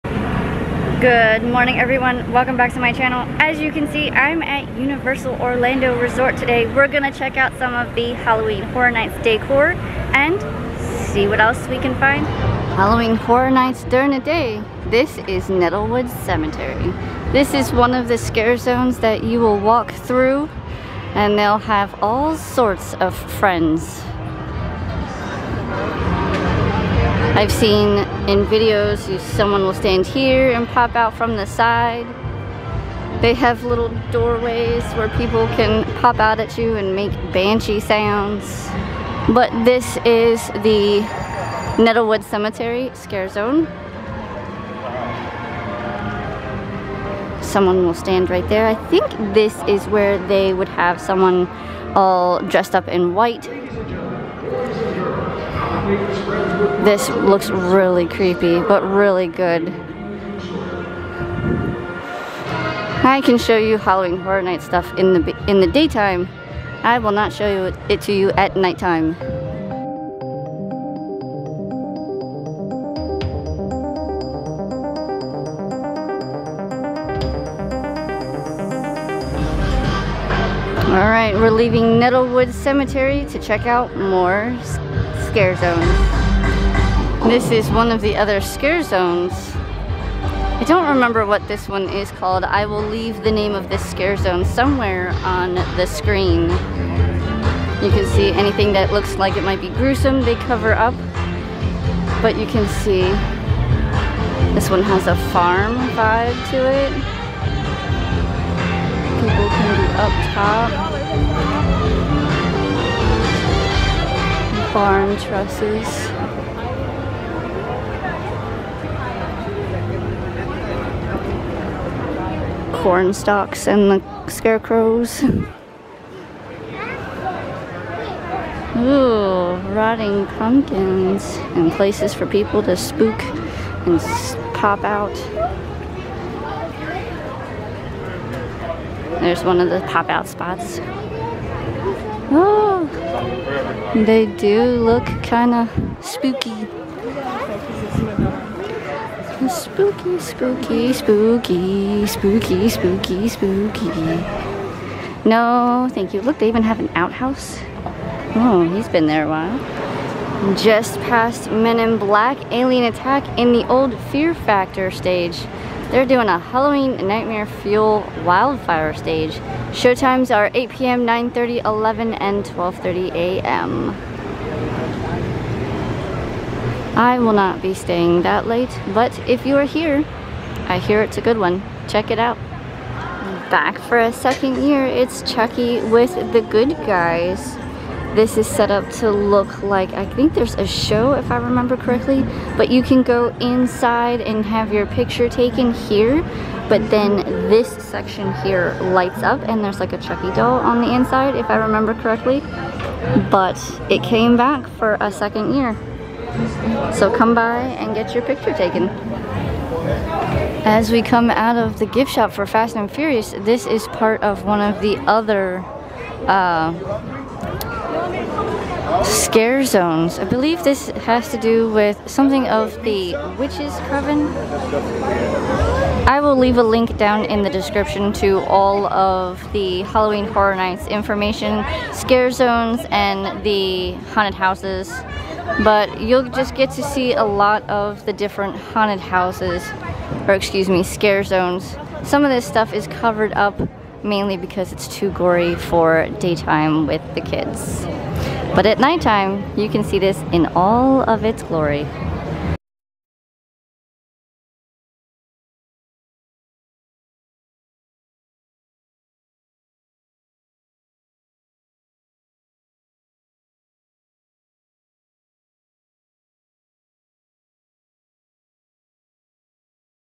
Halloween Horror Nights Decor during the Day!
Hello Everyone!
In today's video, I take you along with me as I walk around and look at the Halloween Horror Nights Props and decorations at Universal Studios park. This video contains some of the scare zones throughout the park.

Scare Zones Shown (in order):
1. Graveyard: Deadly Unrest (Nettlewood Cemetery)
2. Scarecrow: Cursed Soil
3. Halloween Nightmare Fuel Wildfire Show
4. Good Guys Photo Op
5. Conjure the Dark

That is all for me in this video. I'll see you in my next video.